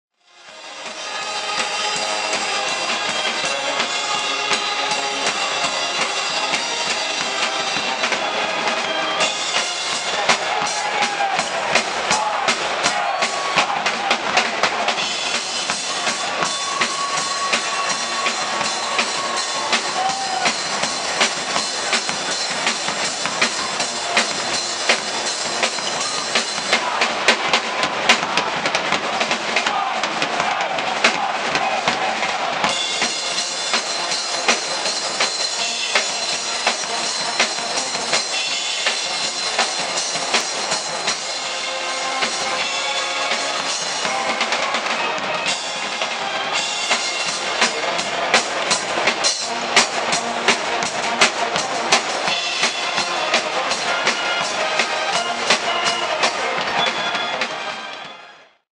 2015 Pitt vs Purdue Hoops Dec 1 with Pitt Band and Cheer
A little Pitt Pep Band action at the Dec 1 Pitt vs Purdue game